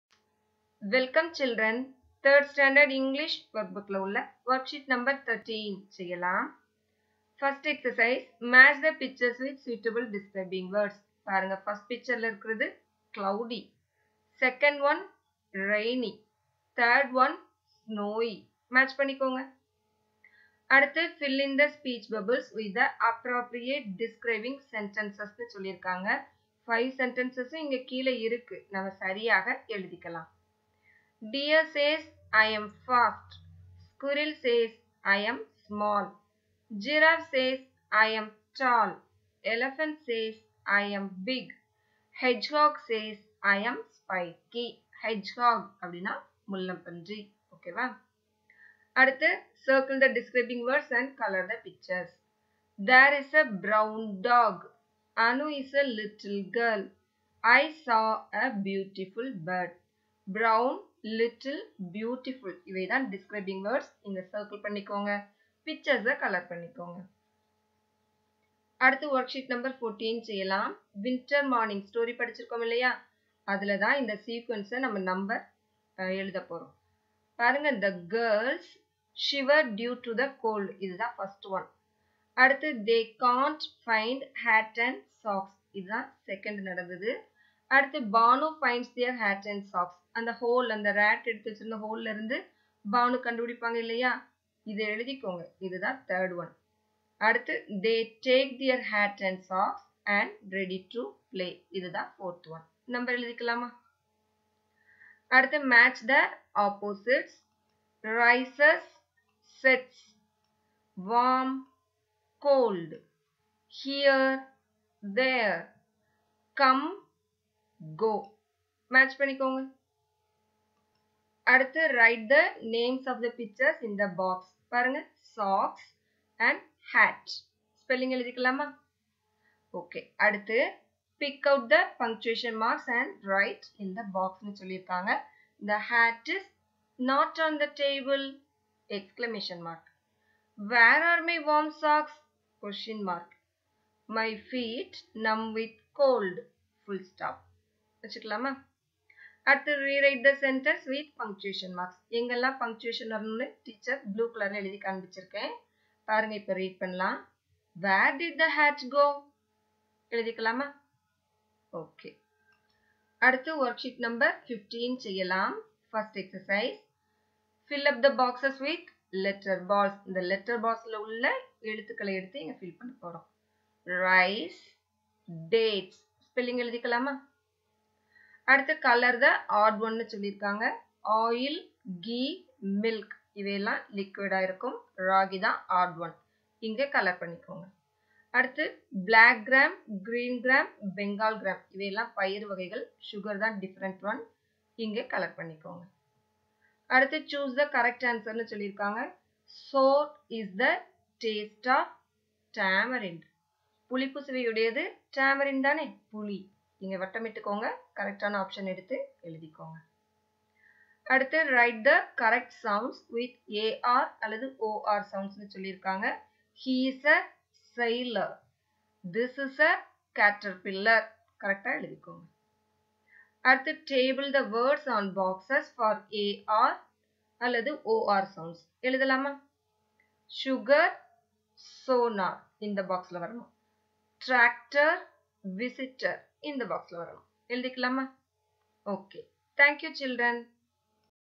Std 3 - English - Worksheets-13,14,15
English workbook - worksheets no.13, 14 and 15 with answers.